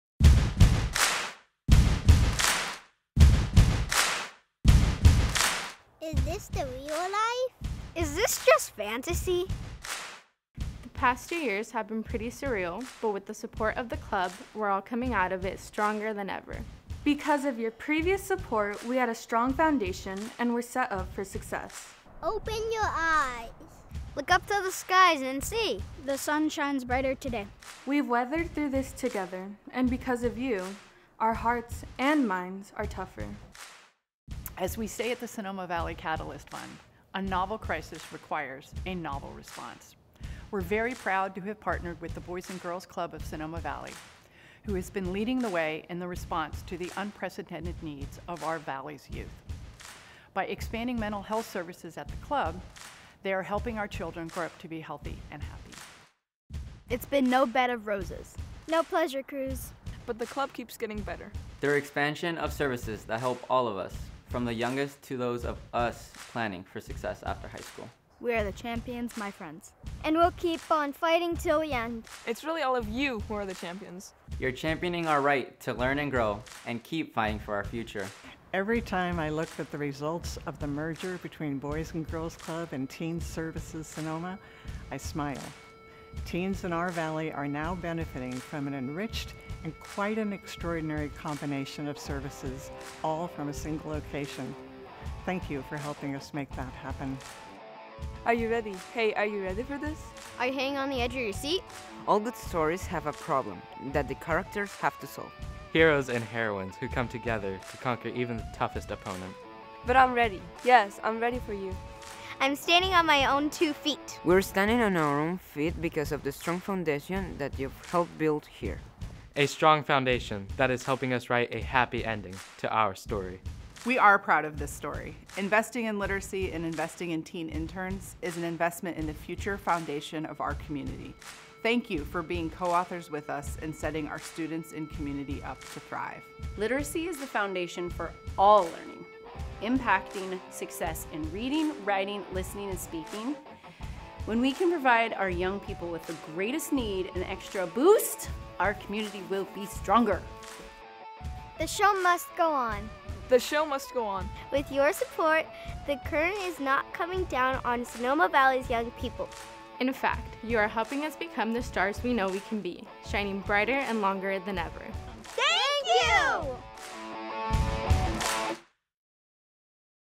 Sweetheart 2022 Fund-A-Need Video
In 2022, Boys & Girls Clubs of Sonoma Valley celebrates 60 years of serving youth in this wonderful community of ours. Thousands of children have been served and a lot has been accomplished over those 60 years, however none of it would have been possible without the support of our Champions. Champions like Helen Fernandez, who is now celebrated outside our Clubhouse, helped found the original Club at the Sonoma Community Center in 1962. Champions like our Dream Builders who helped to build the incredible facility we celebrate in this evening. And each of you, our Champions today, who have helped us not just weather this pandemic, but have empowered us and the kids we serve to come out of this once-in-a-lifetime event stronger than ever. With you as our Champions, in the last two years we have…

Merged with Teen Services Sonoma to establish an enriched and quite extraordinary combination of services for Sonoma teens, all from a single location.
• Stepped up to support the Sonoma Valley Unified School District and our community to host distance learning locations across Sonoma Valley and provide access to school and academic support for kids who might otherwise not have been able to attend school virtually.
• Expanded our Multi-Sensory Literacy program to all elementary and middle school sites to support reading and learning across our Valley.
• Added Mental Health Services to help address the dramatic unmet need, which was only exacerbated by recent fires and the pandemic.
• Added an eighth site at El Verano Elementary School to serve even more kids in our community.
• Improved our ratio of staff to kids to 1:16 in an effort to provide even better academic, social, and emotional support to the members in our care.

With you as our Champions, in the next year we will be able to…
• Provide 1:1 and group counseling to 150 Sonoma Valley kids and teens, at no additional cost to our families.
• Help Sonoma Valley students regain any academic ground lost during the pandemic and keep their dreams on track.
• Empower our staff to continue being local and national leaders in the areas of safety.
• Expand our team to meet the growing need of our programs and services.
• Maintain our very successful Teen Internship program, offered in partnership with the Sonoma Valley Education Foundation, to provide valuable work experience to teens and greater academic and social/emotional support to our younger members.

Thank you for being Champions for kids in Sonoma Valley. None of our success would be possible without each and every one of you. We are exceptionally grateful that you entrust us to be your partners to affect positive change in the valley we all love and we look forward to much more shared success in the future.